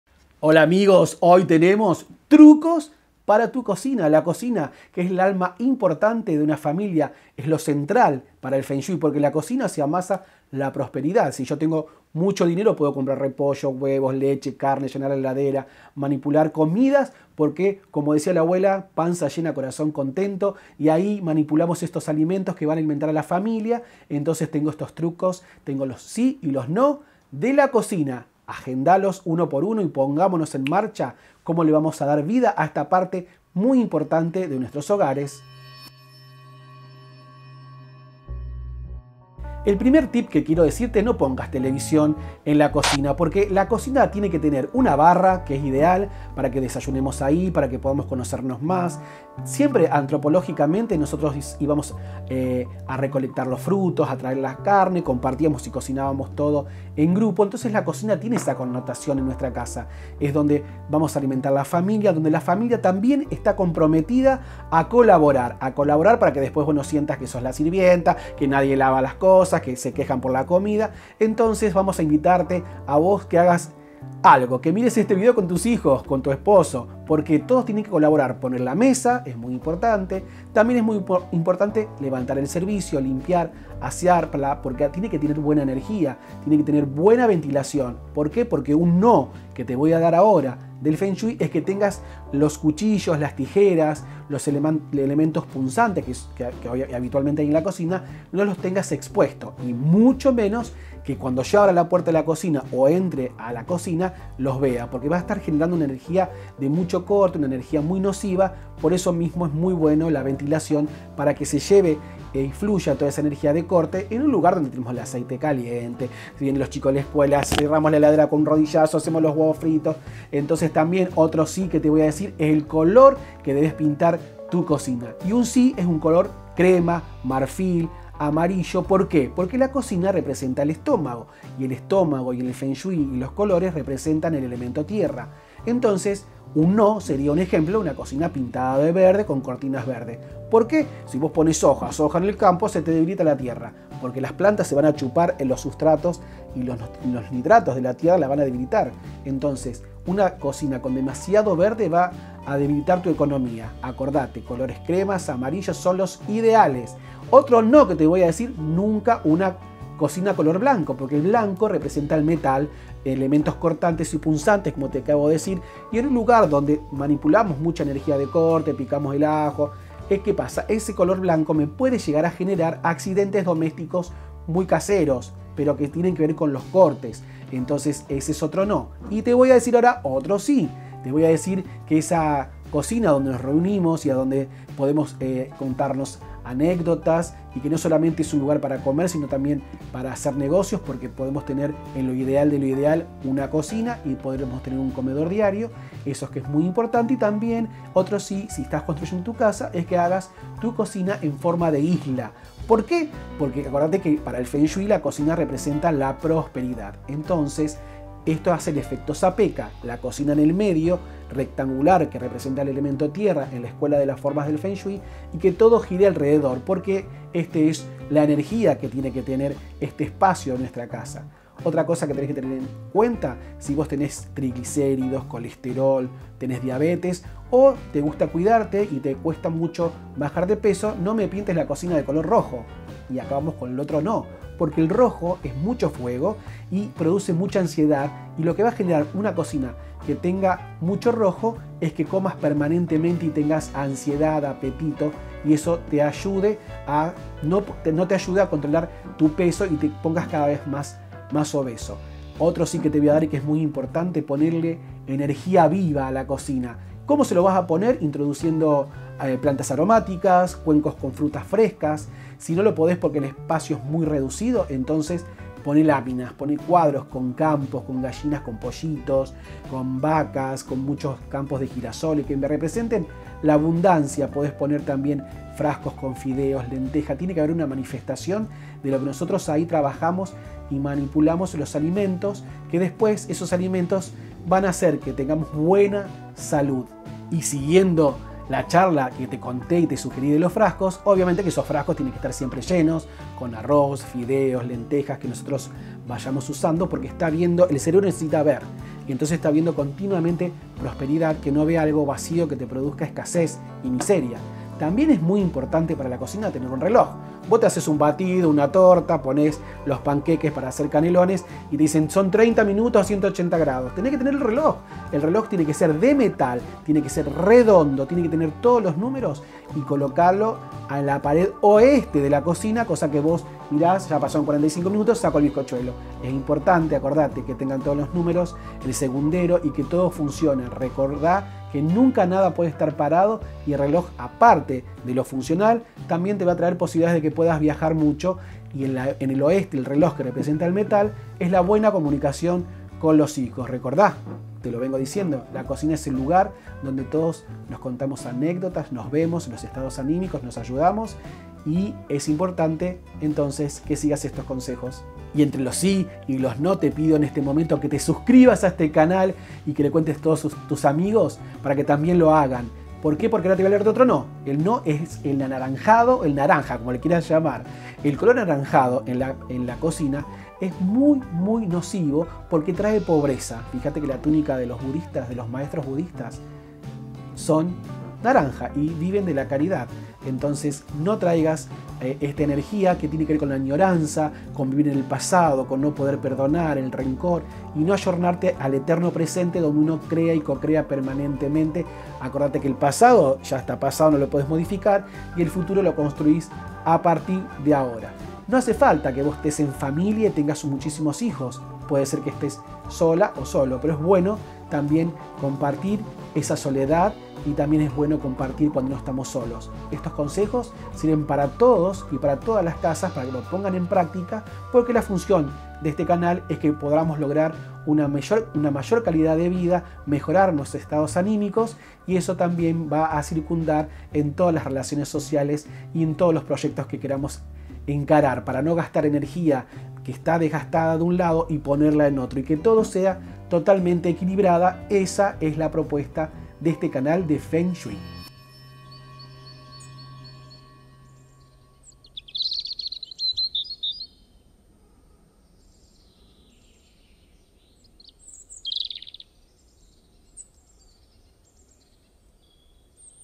🍳 Feng Shui en la COCINA: prosperidad y abundancia (Gustavo Antunez, Maestro de Feng Shui)
Para el Feng Shui, la cocina simboliza la prosperidad y hoy te doy muchos trucos para mejorarla (y también lo que NO debes hacer).

Gustavo Antunez, Maestro de Feng Shui